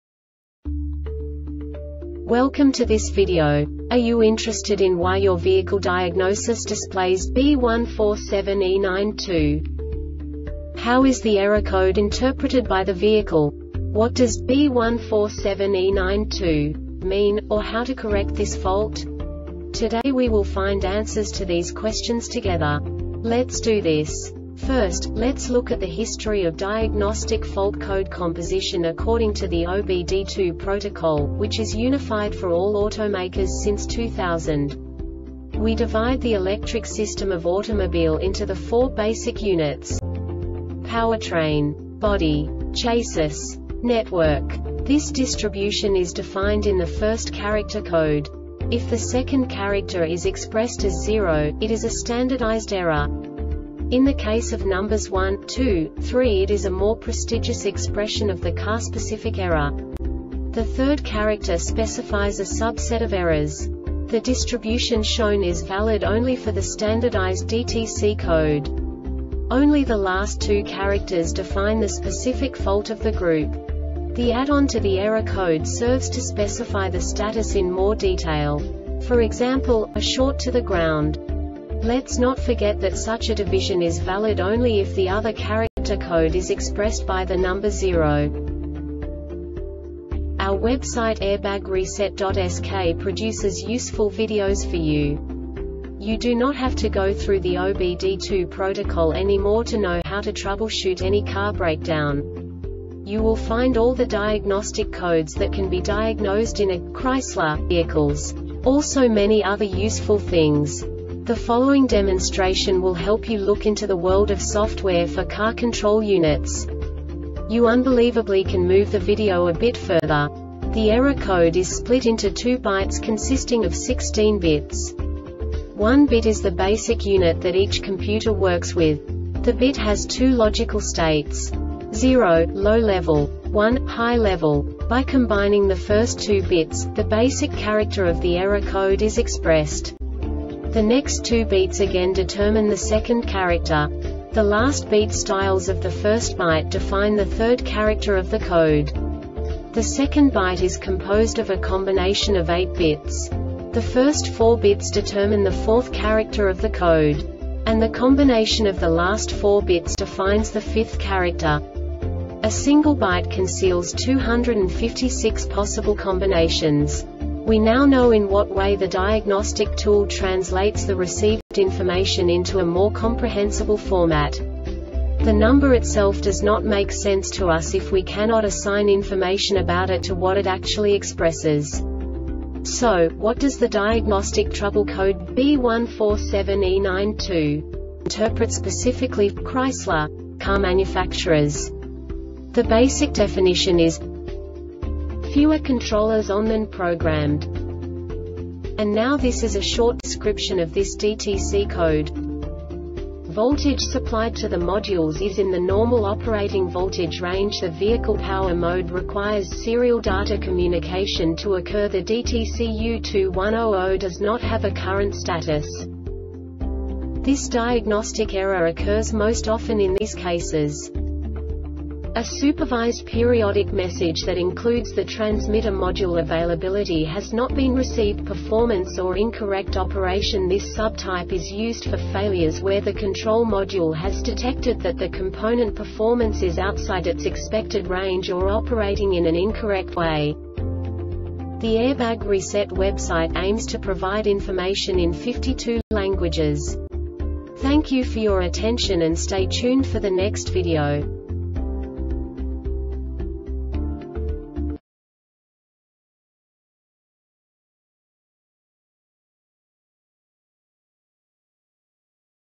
DTC Chrysler B147E-92 Short Explanation
Contents:
0:21 Basic DTC analysis according to OBD2 protocol standard.
1:48 Insight into programming
2:58 A diagnostically understandable description of the Diagnostic Trouble Code
3:05 Basic error definition

Make:
Chrysler
Code:
B147E 92
Definition:
Fewer Controllers On Than Programmed
Description: